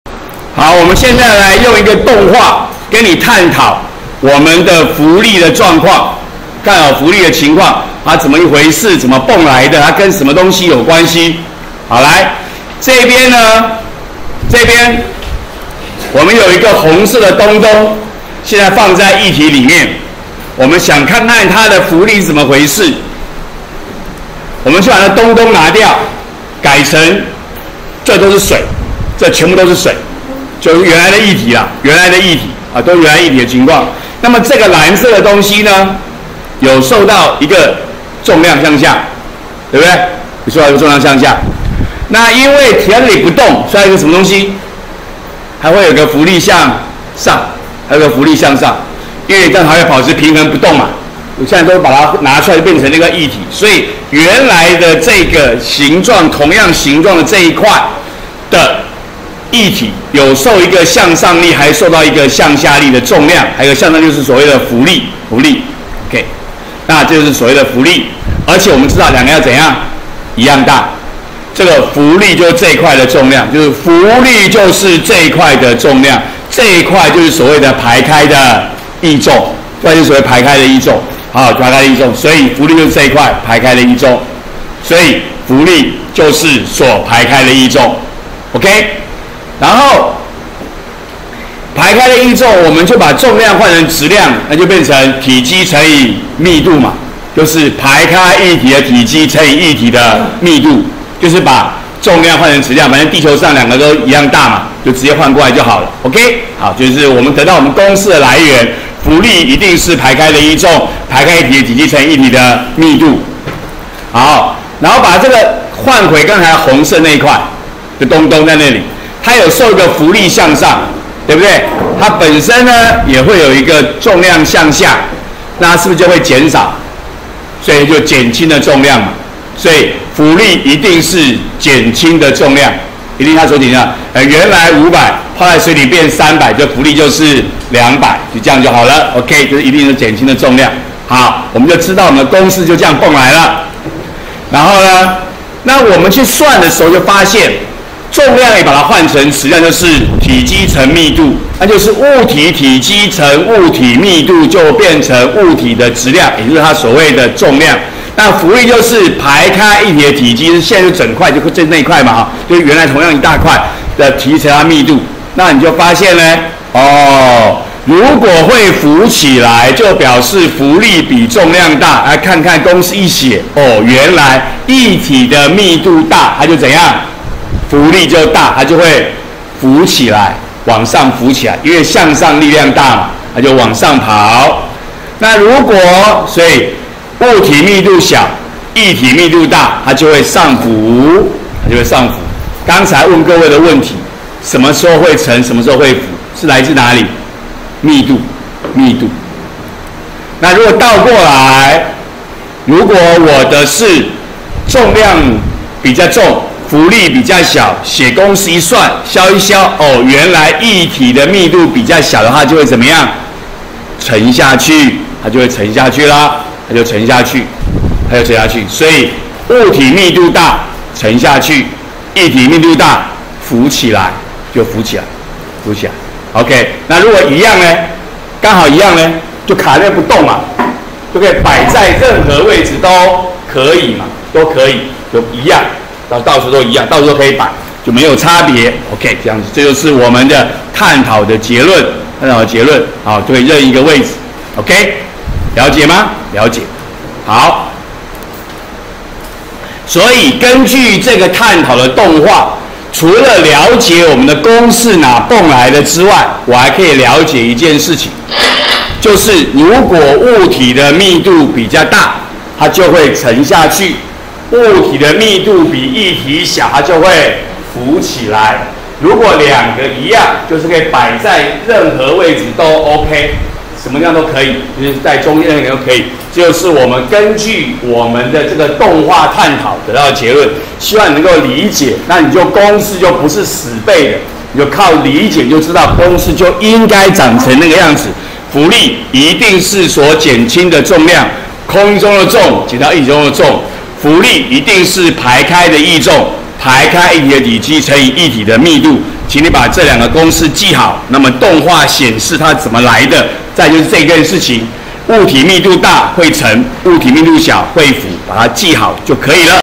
104學年國二理化＿常見的力33：浮力探討動畫；密度與沉浮
以動畫說明浮力原理（浮力公式）如何而來。藉由分析知道影響沉浮的因素是密度。